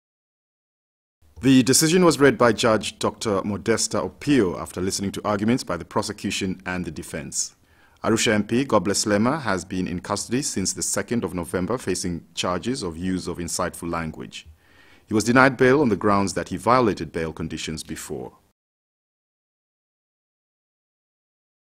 High court agrees to listen to case appealing denial of bail to Arusha MP Godbless Lema.
The high court in Arusha has agreed to listen to a case appealing decision of a court to deny Arusha MP Godbless Lema bail.